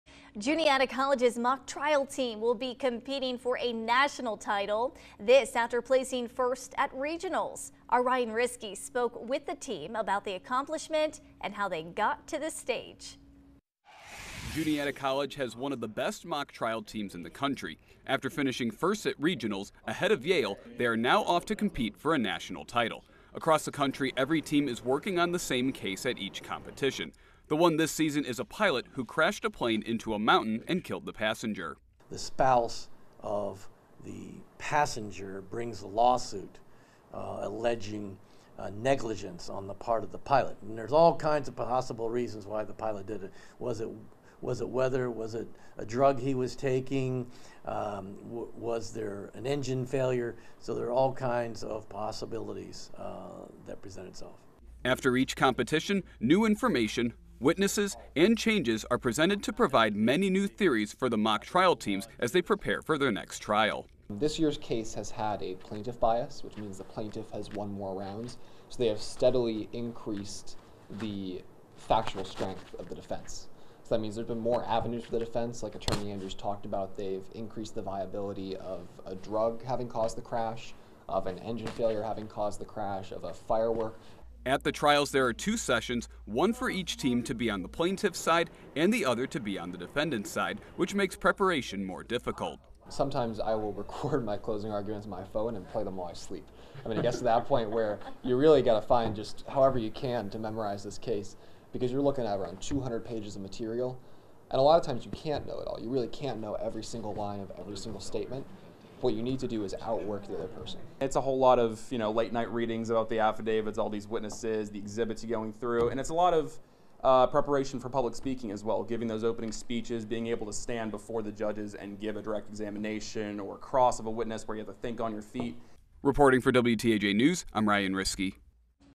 Juniata College's mock trial team heads to nationals
Juniata College's mock trial team won their regional and is heading to nationals.